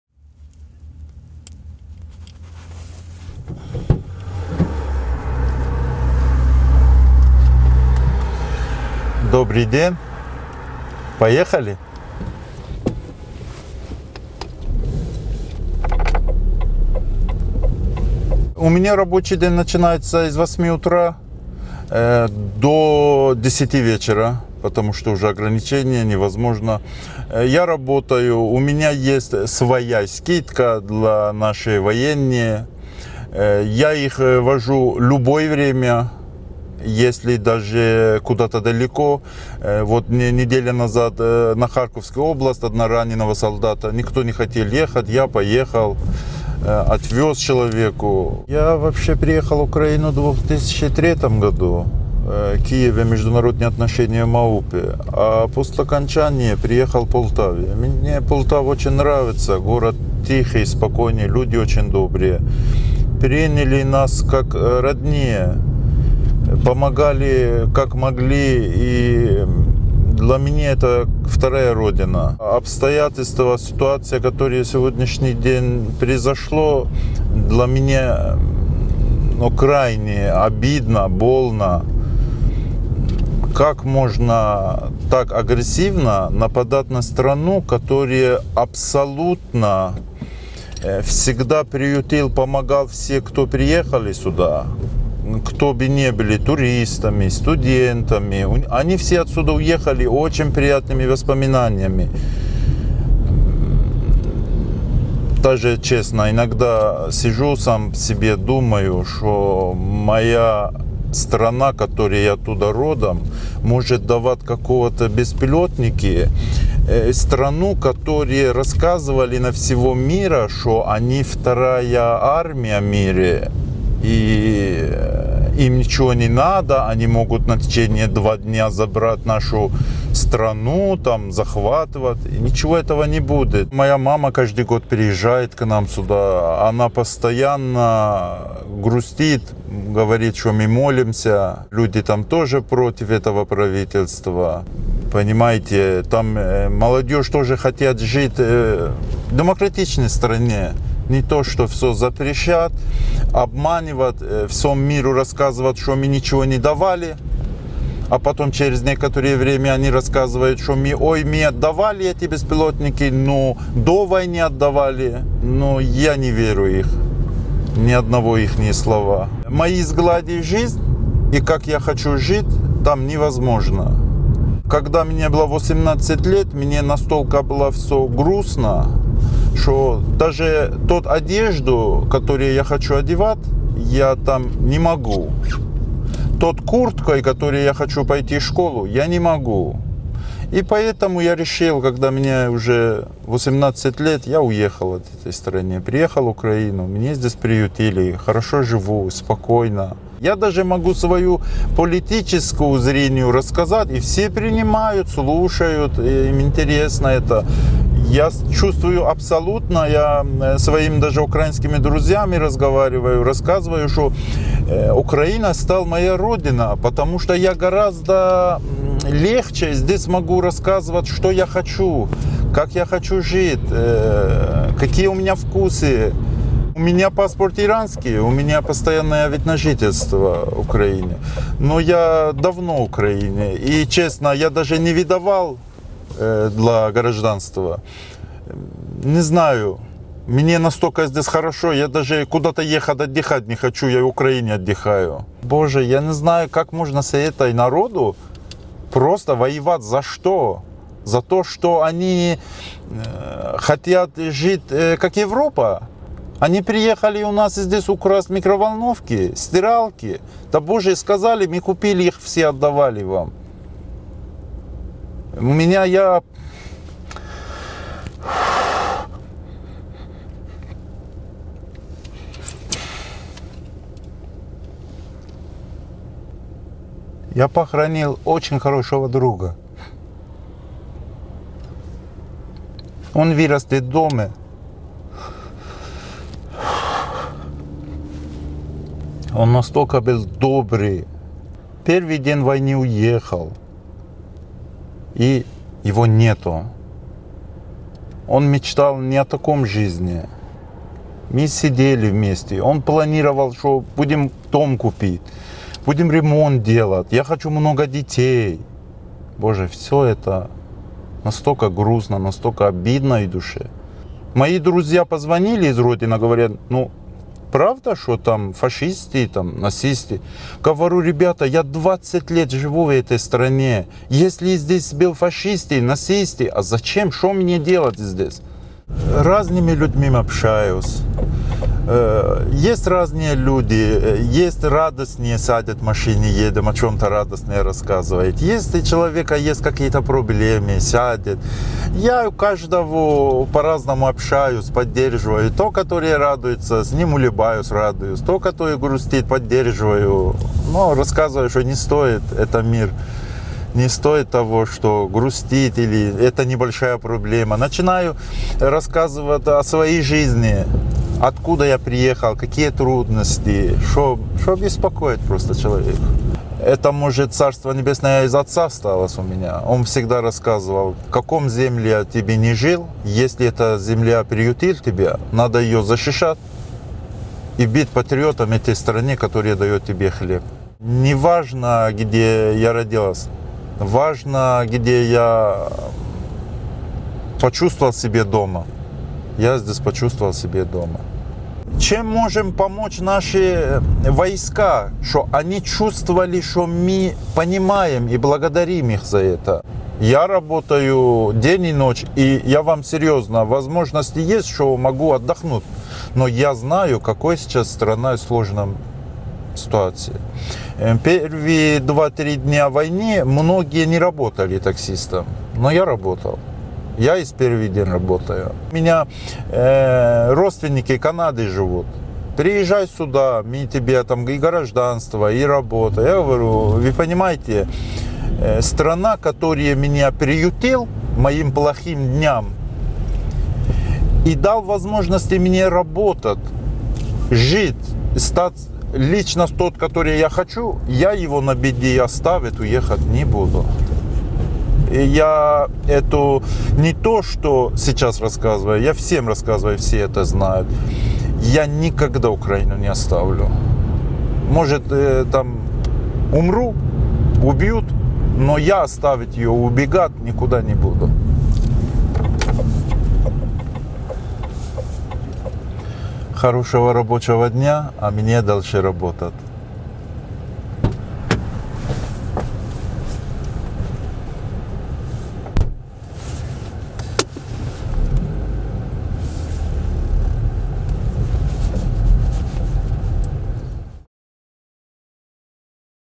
"Я тут відчув себе вдома" — іранець про життя в Полтаві
Іранець Амір Вар приблизно 20 років мешкає в Україні. Каже, що 2003-го переїхав із міста Мераге, бо хотів жити в демократичній країні. У Полтаві чоловік має кілька бізнесів і працює таксистом.
Що думає про батьківщину, яка  надала дрони-камікадзе для Росії, чому завжди в машині має прапор України і як допомагає військовим, дивіться далі.